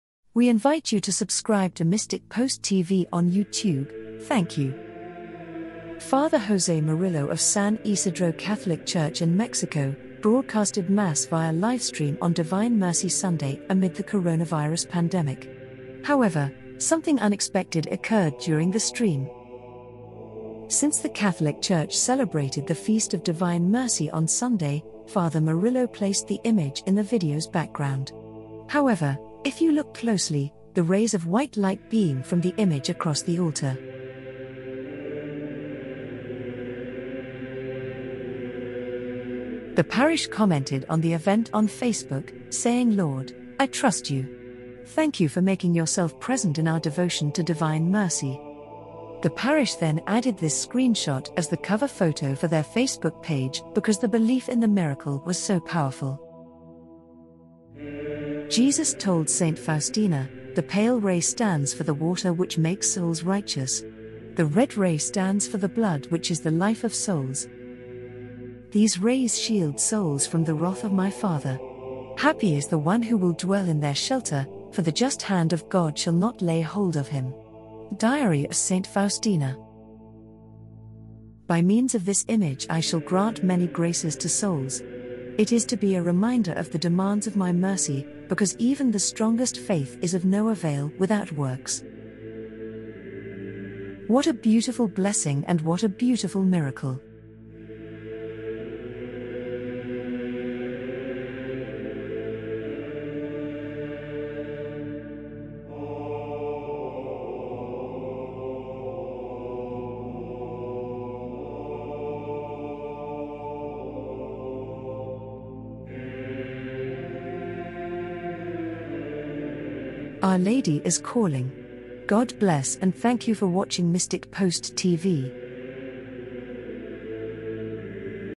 DIVINE MERCY MIRACLE? LIGHT BEAMS FROM DIVINE MERCY IMAGE-VIRAL EVENT OCCURS DURING MASS LIVE-STREAM
Father José  Murillo of San Isidro Labrador Catholic Church in Mexico broadcasted Mass via livestream on divine mercy Sunday amid the coronavirus pandemic. However, something unexpected occurred during the stream.

Since the Catholic Church celebrated the feast of Divine Mercy on Sunday,  Fr. Murillo placed the image in the video’s background. However, if you look closely, the rays of white light beam from the image across the altar.

Jesus told St. Faustina, “The pale ray stands for the Water which makes souls righteous. The red ray stands for the Blood which is the life of souls.”
“These rays shield souls from the wrath of My Father. Happy is the one who will dwell in their shelter, for the just hand of God shall not lay hold of him.” (Diary of St. Faustina, 299)
“By means of this image I shall grant many graces to souls. It is to be a reminder of the demands of My mercy, because even the strongest faith is of no avail without works.” (Diary of St. Faustina, 299)
What a beautiful blessing!